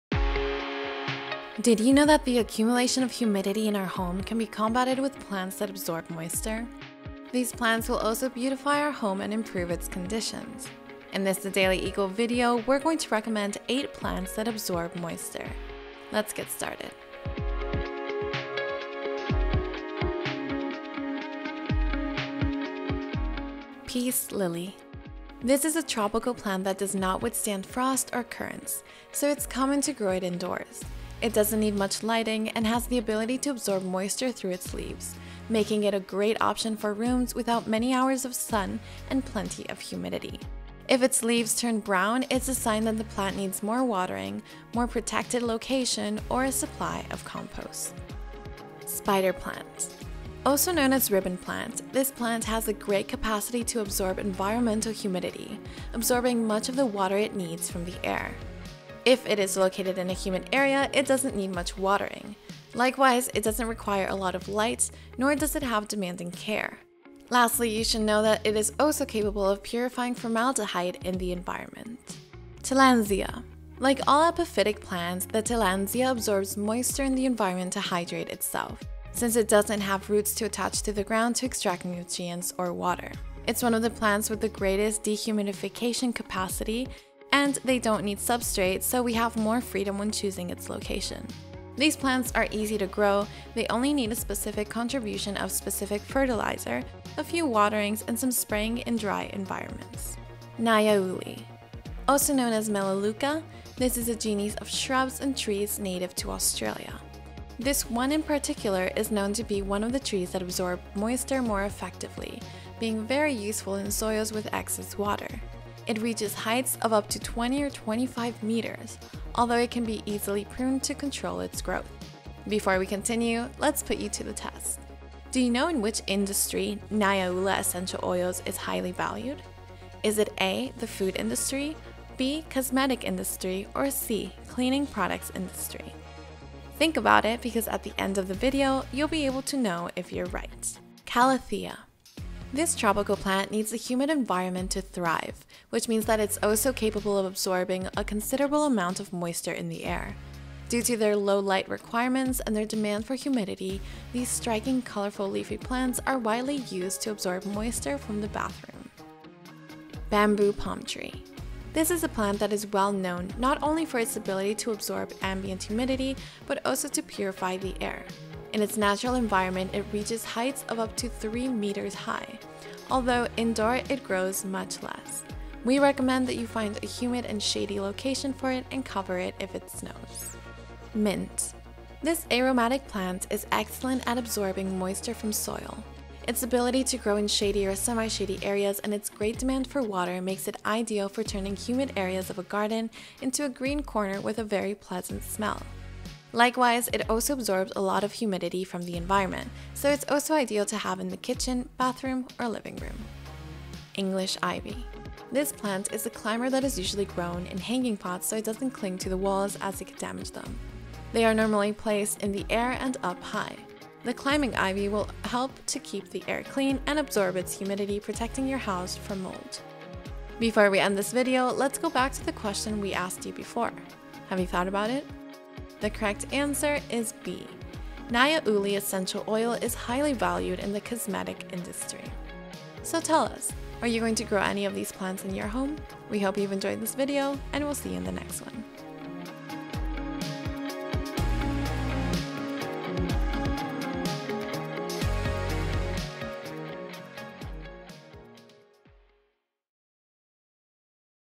8 PLANTS that ABSORB MOISTURE 🌿💧 Names and their care!
🌾💦 Do you want to GROW PLANTS that ABSORB environmental HUMIDITY? In this The Daily Eco video we're going to recommend 8 DEHUMIDIFYING PLANTS to have at home. Discover how to decrease the HUMIDITY in your home with these INDOOR PLANTS! 🌿